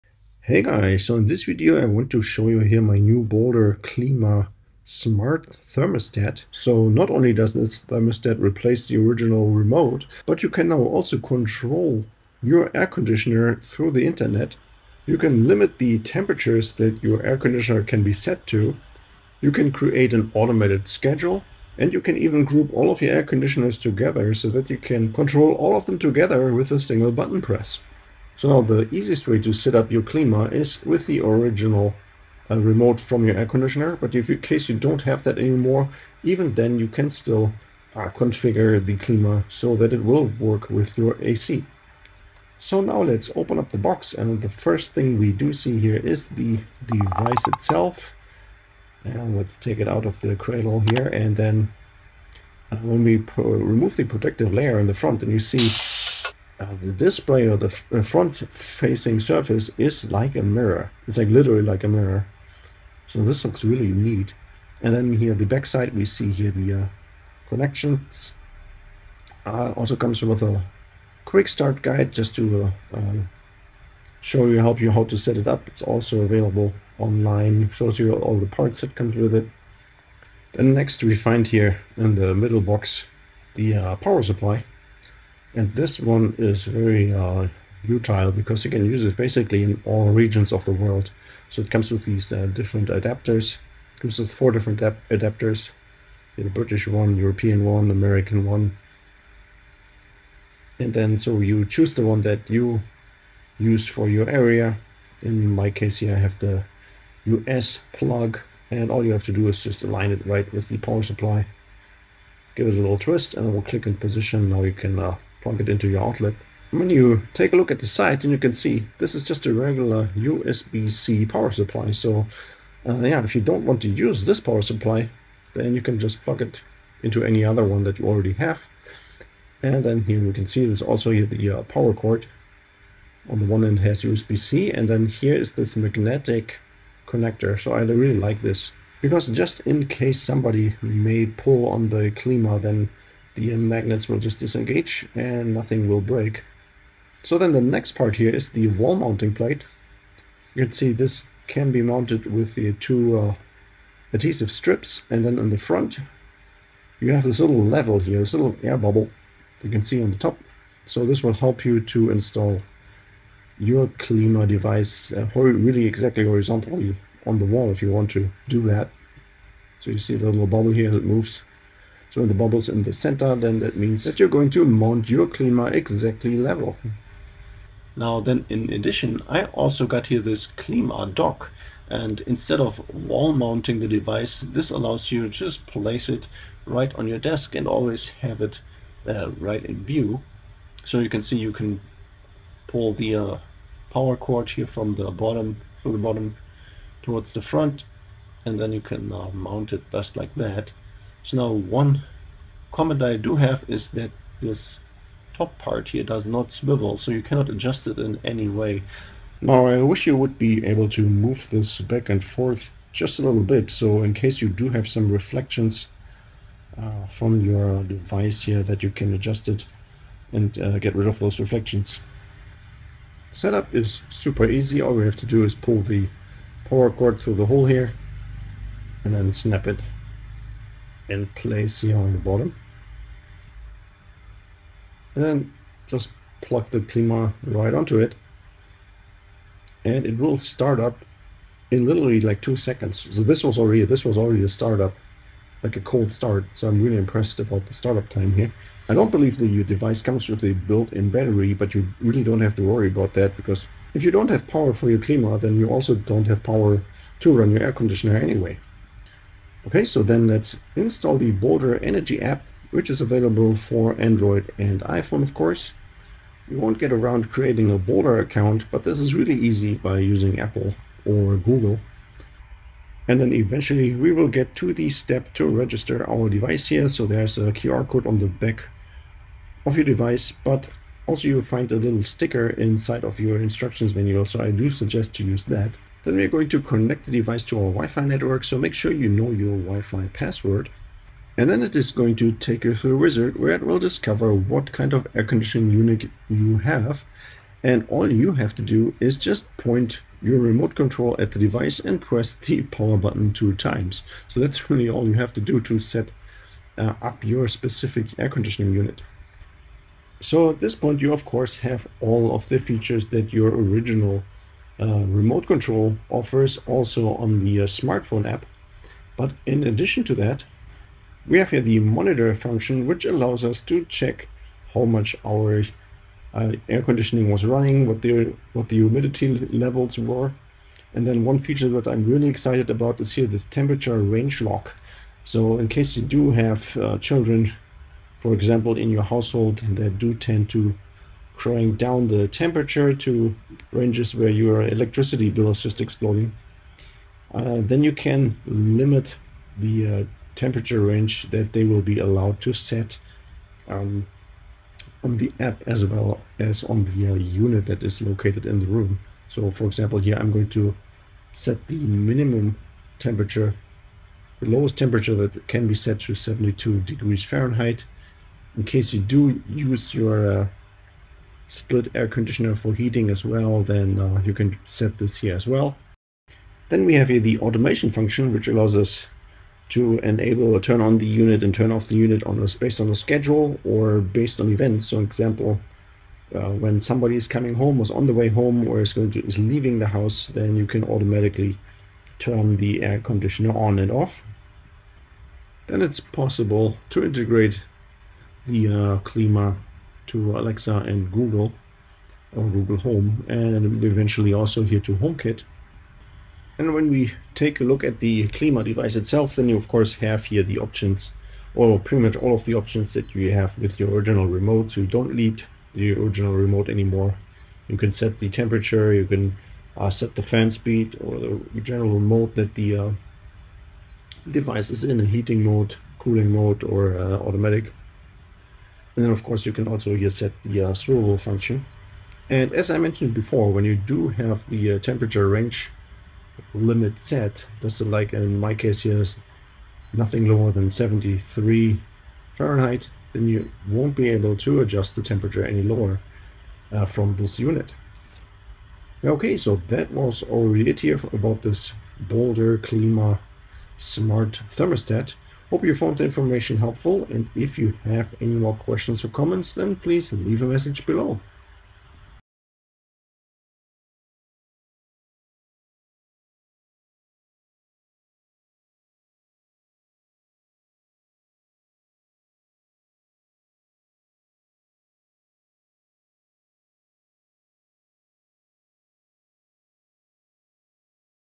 Boldr Klima Smart Thermostat for Air Conditioners (Unboxing, Installation how to)
In this video we are taking a look at our new Boldr Klima Smart A/C Thermostat. We are going through an unboxing as well as the entire setup sequence.
The Boldr Klima Smart Thermostat allows us to control our A/C unit through the internet. It also offers many other features, which are not available with the regular A/C infrared remote.Once of our favorite features is the "Dad mode", which allows us to limit the temperature range, so someone cannot set the A/C unit to an extremely cold temperature anymore. This will allow us to save on our energy bill.It is also possible to set up automated schedules or control the A/C unit based on other events.

The Boldr Klima at Amazon